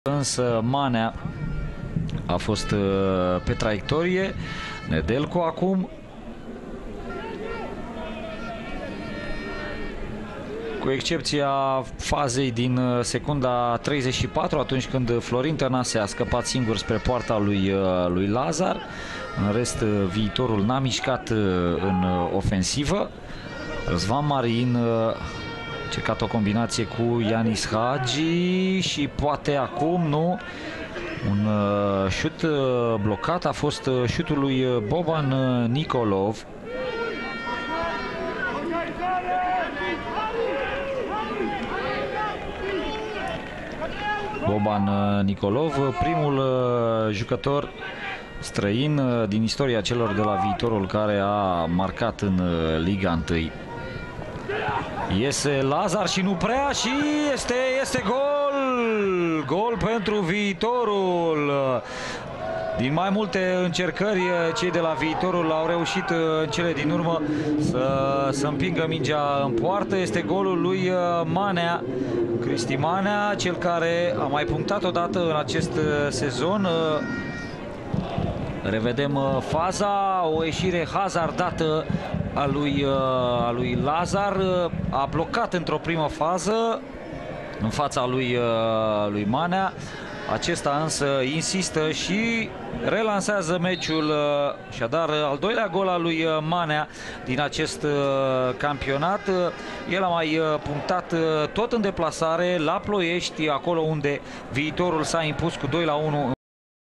Cristi Manea inscrie la Botosani in minutul 30
2-1 pentru gazde